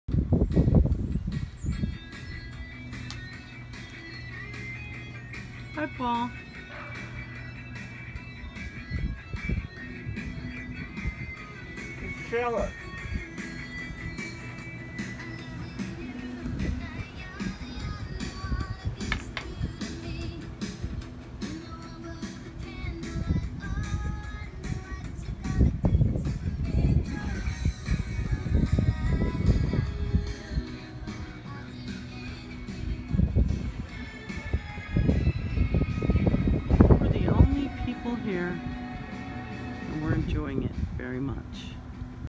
Favorite Marina View Bar
Gone but not forgotten a favorite of boaters and live aboards with an awesome view and never over crowded - random day during the week.